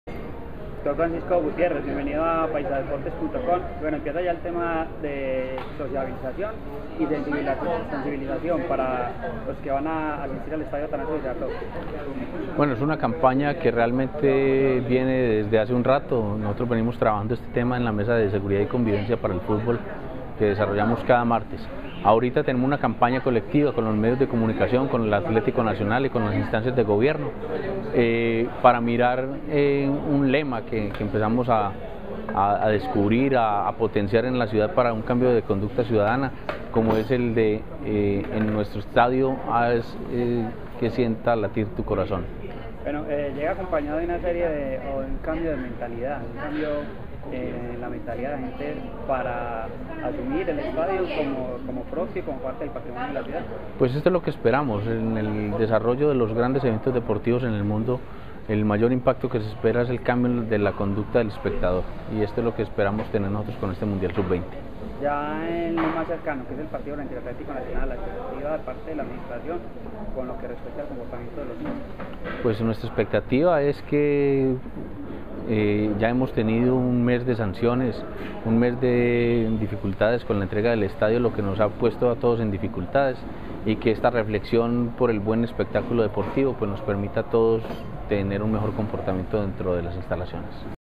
Francisco Gutierrez-INDER Medellin
El director del INDER de Medellin, habla acerca de la nueva cultura para disfrutar del estadio Atanasio Girardot